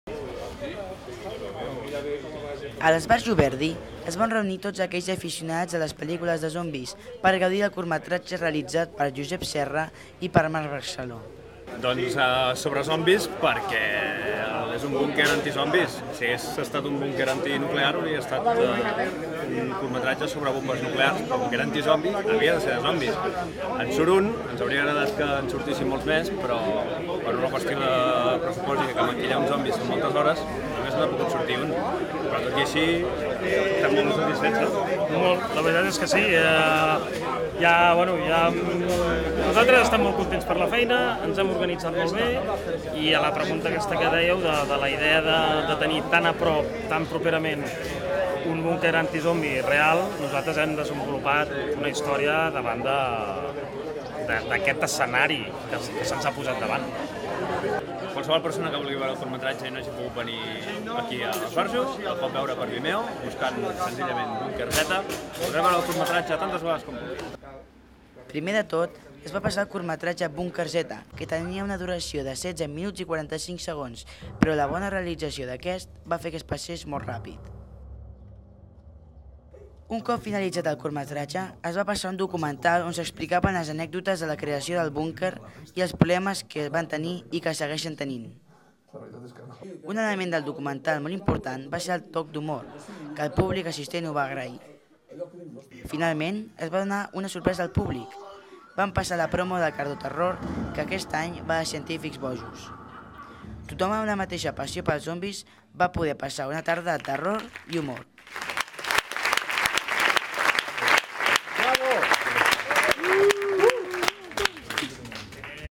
Zombies cardedeuencs
El búnquer anti-zombis, situat a Cànoves i Samalús, ha mostrat al públic quines dificultats i sensacions s'han trobat al llarg de la seva construcció. El dijous passat el famós BúnquerZ va aparèixer a la gran pantalla el passat dijous a l'Esbarjo.